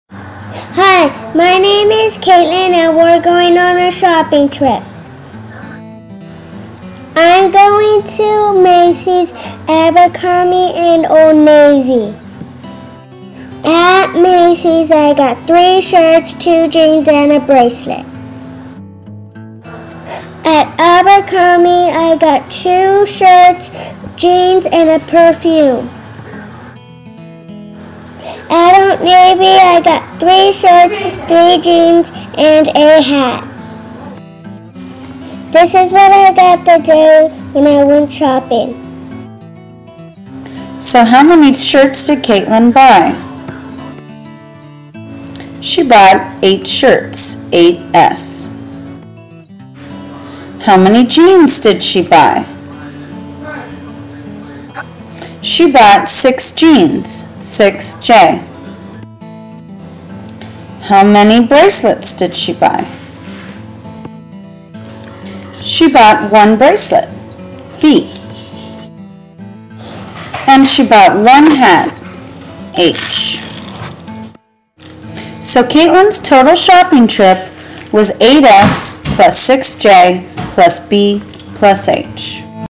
Combining Like Terms
This lessons uses a shopping trip to explain combining like terms